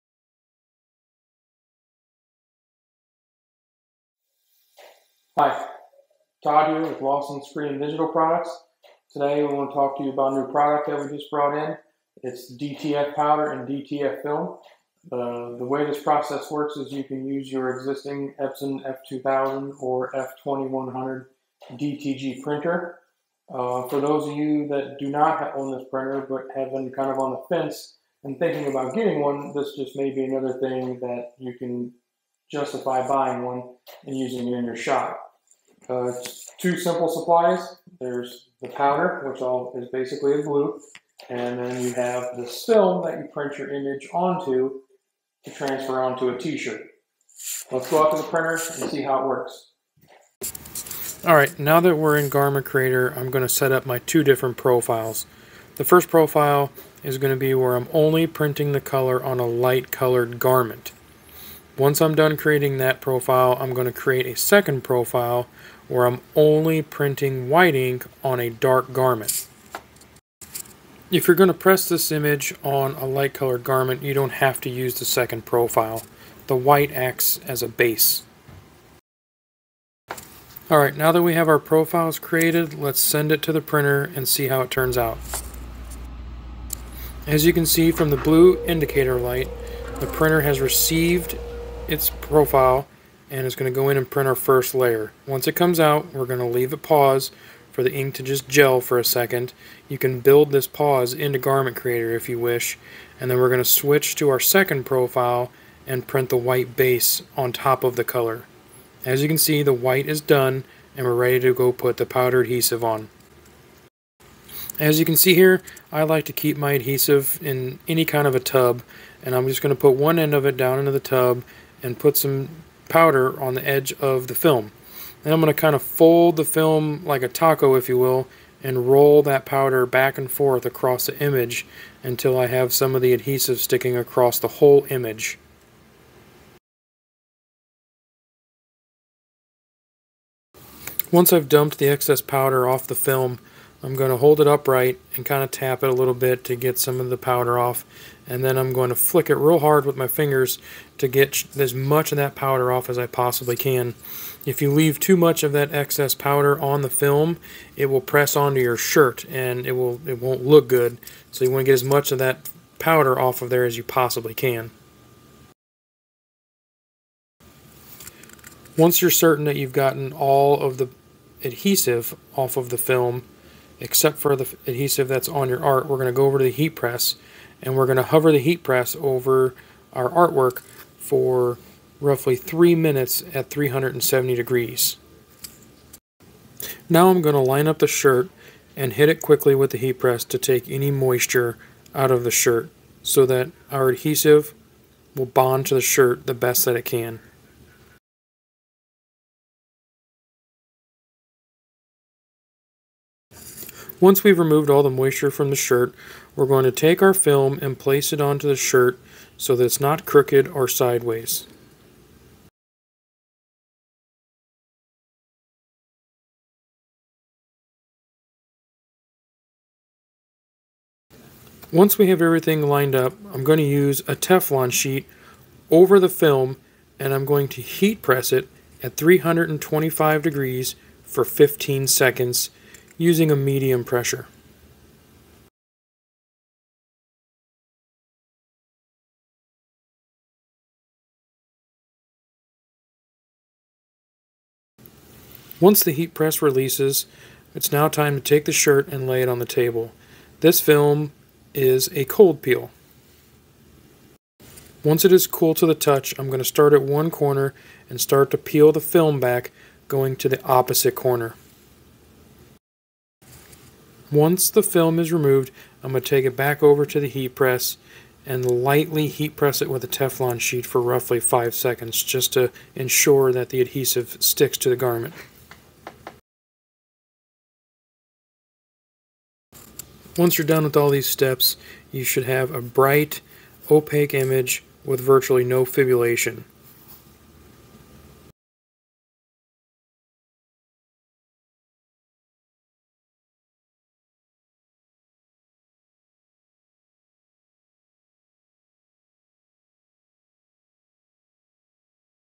How to Print Direct-to-Film (DTF) Transfers on Your Epson Direct-to-Garment (DTG) Printer
This is an instructional video on how to print direct to film (DTF) transfers on an Epson F2100 DTG printer using Garment Creator. In this video, Todd from Lawson Screen & Digital Products reviews the reasons for using DTF over DTG and then goes through the step-by-step instructions for printing DTF heat transfers. You will first learn how to set up color profiles in Epson's Garment Creator for the reverse printing needed for DTF. Then, see tips for applying and drying the adhesive DTF powder on your transfer film. Finally, Lawson will show you the best tips for getting great color brilliance and wash fastness (adhesion) to your shirt.

0:00 INTRO
0:51 GARMENT CREATOR
1:25 PRINTING ON FILM
1:53 APPLYING POWDER
2:55 USING THE HEAT PRESS
4:08 REVEAL

Learn how to screen print, or keep up with the latest silk screen printing equipment and techniques by watching Lawson TV